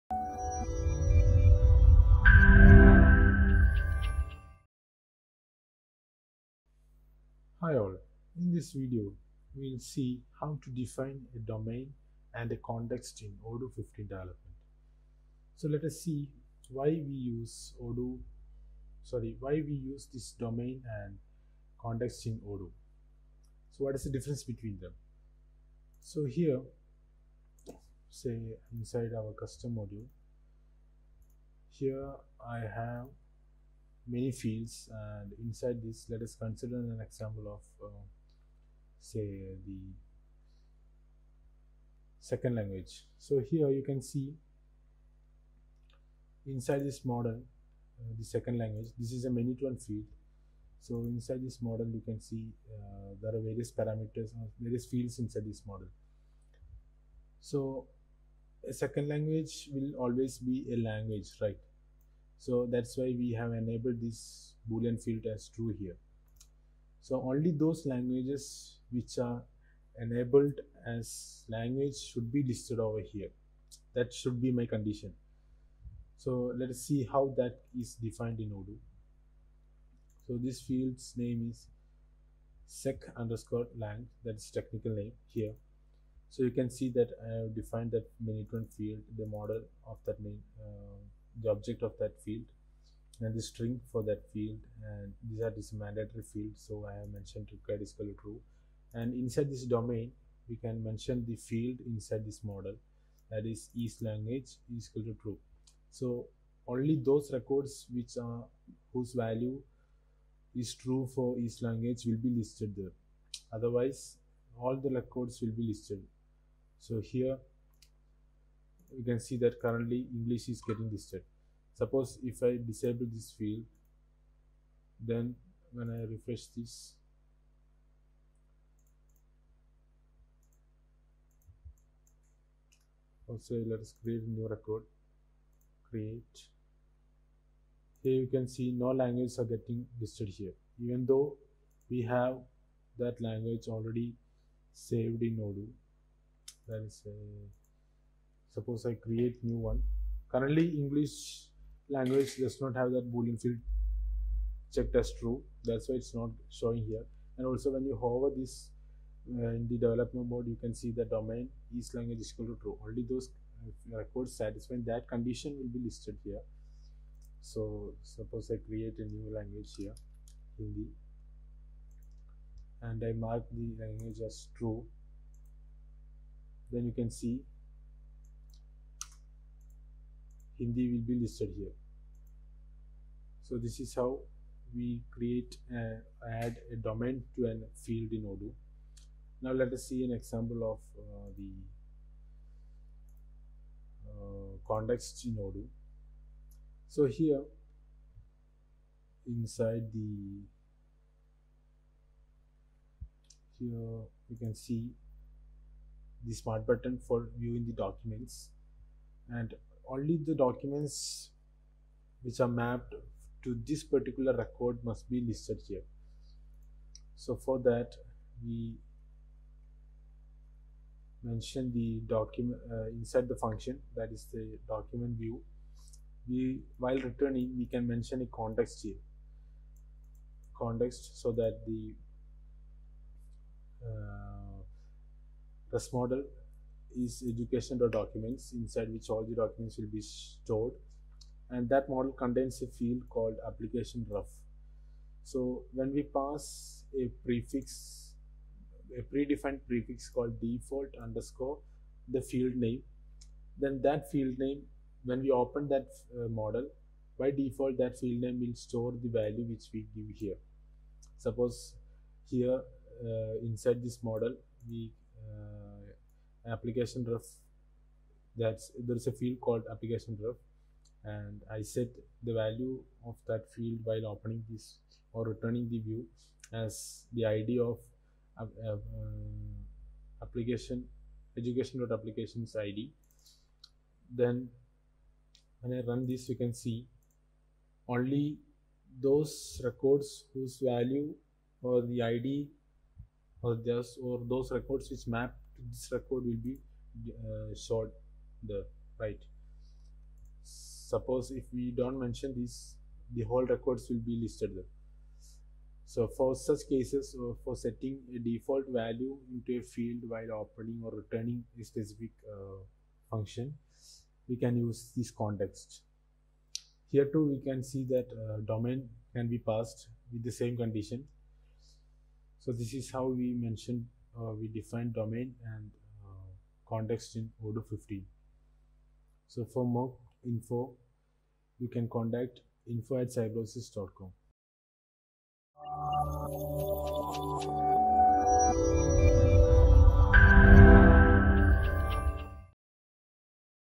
How to Add a Context and Domain in Odoo 15 | What Is a Context in Odoo | Odoo Development Tutorials
In this video, we will discuss How to add a context and domain in odoo15. The context in Odoo is a part of the environment of a record set.

Video Contents
00:05 Introduction
00:40 Adding a domain
03:15 Adding a context

It is basically a python dictionary that carries the session data. This data can be used on both the client-side user interface and also the ORM business logic in the case of the server-side.

So most importantly we can use the context to pass the specific data to a function in odoo. You may have seen that almost all methods in odoo use a context parameter.

Odoo domain is used to select records from a model or database table.  It is a very common use case when you need to display a subset of all available records from an action, or to allow only a subset of possible records to be the target of a many2one relation.

The way to describe these filters in Odoo is called a domain. It can be defined as a list of operations that are used to filter your data or for searching.